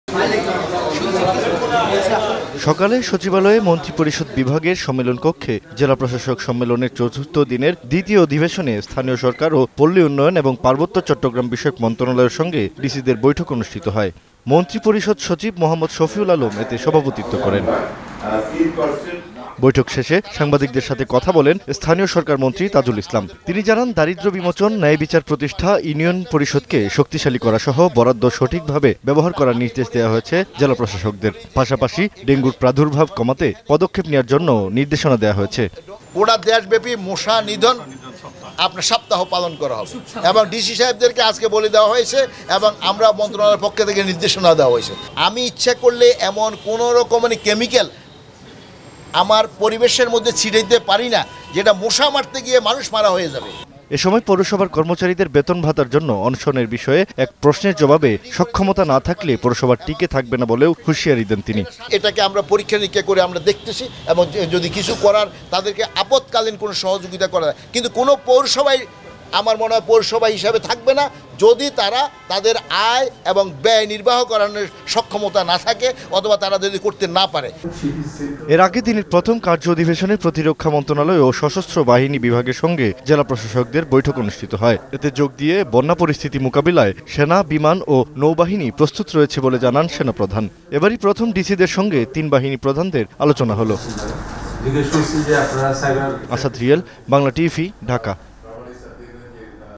আয়-ব্যয় নির্বাহ করতে না পারলে পৌরসভা থাকবে না: তাজুল
🔹 ID – Asad Real

🔹 ID - iamasadreal

আগামী ২৫ থেকে ৩১ জুলাই দেশব্যাপী মশক নিধন সপ্তাহ পালন করা হবে বলে জানিয়েছেন স্থানীয় সরকার মন্ত্রী তাজুল ইসলাম।  এ বিষয়ে প্রয়োজনীয় ব্যবস্থা নিতে জেলা প্রশাসকদের নির্দেশনা দেওয়া হয়েছে বলেও জানান তিনি।  সকালে সচিবালয়ে মন্ত্রিপরিষদ বিভাগের সম্মেলন কক্ষে জেলা প্রশাসক সম্মেলনের চতুর্থ দিনের দ্বিতীয় অধিবেশন শেষে তিনি এ কথা জানান।  এর আগে জেলা প্রশাসক সম্মেলনে, দেশের বন্যা পরিস্থিতি মোকাবিলায় তিন বাহিনী প্রস্তুত রয়েছে বলে জানান সেনাপ্রধান মেজর জেনারেল আজিজ আহমেদ। আসাদ রিয়েলের রিপোর্ট।